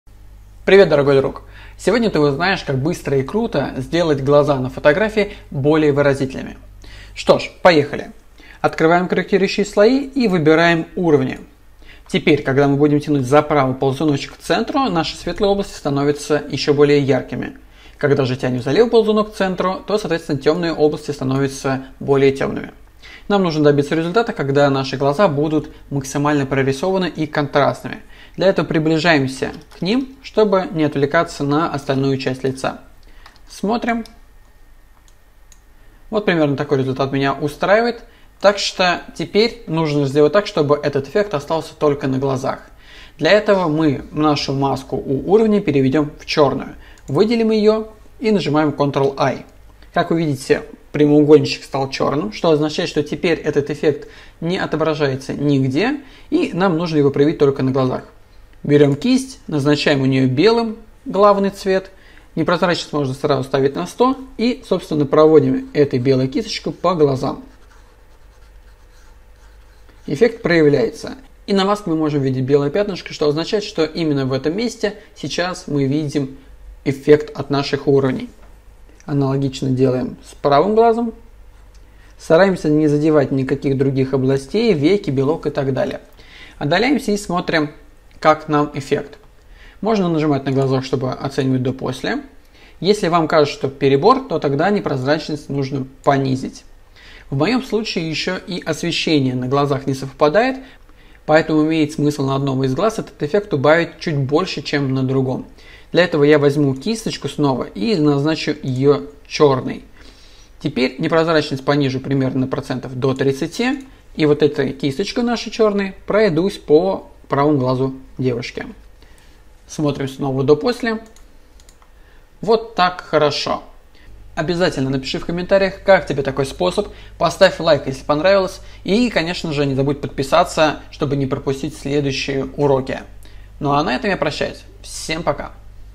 Как сделать глаза более выразительными! Быстро и легко
Урок по обработке глаз в фотошопе. Очень простой, но эффективный метод сделать глаза ярче.